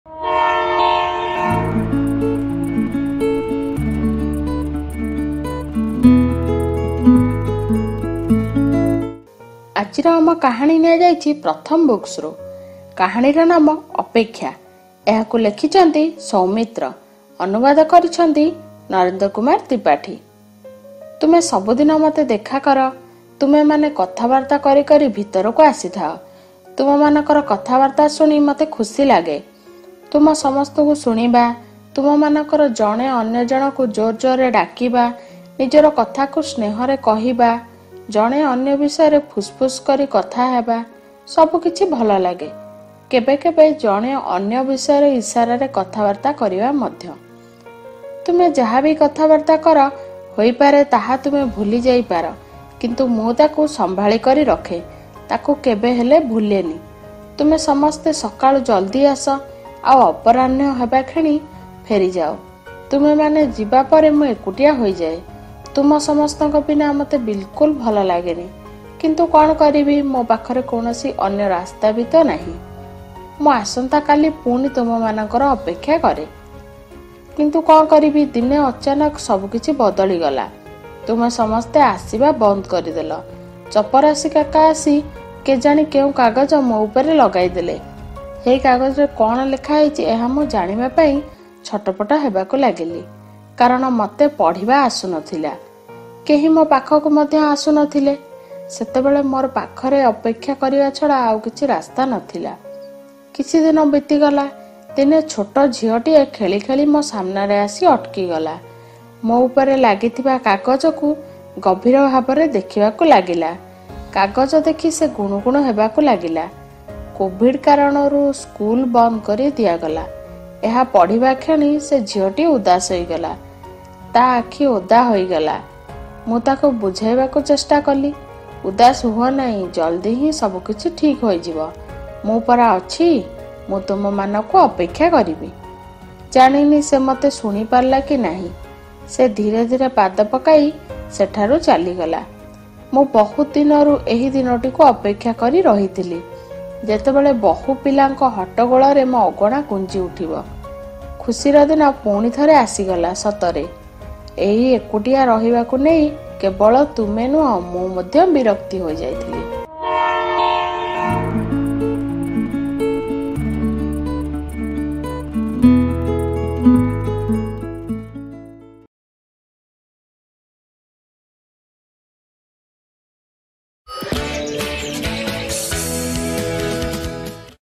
Apekhya (Odia) I ଅପେକ୍ଷା
"ଆସିଗଲା କାହାଣୀ ଟ୍ରେନ୍ ସିଜନ୍ 3 । କାହାଣୀ ଟ୍ରେନ୍ ଅର୍ଥାତ୍ ପିଲାଙ୍କ କାହାଣୀ ସିଧାସଳଖ ଆପଣଙ୍କ ଫୋନ୍ ଯାଏଁ । ଏହି ପ୍ରୟାସ ଆଜିର ଯୁଗର ପିଲାଙ୍କୁ ଘଟଣା - କାହାଣୀ ଏବଂ ବଡ଼ଙ୍କୁ ସେମାନଙ୍କ ପିଲାଦିନ ସହିତ ଯୋଡିବା  ଅଟେ ।
"କାହାଣୀ -  ଅପେକ୍ଷା
ଲେଖକ -  ସୌମିତ୍ର
ପ୍ରକାଶକ -  ପ୍ରଥମ ବୁକ୍ସ୍
ପ୍ରସ୍ତୁତି   -    ପୁର୍ବାଷା"

Created by Pratham and inspired by Rekhta and Nayi Dhara."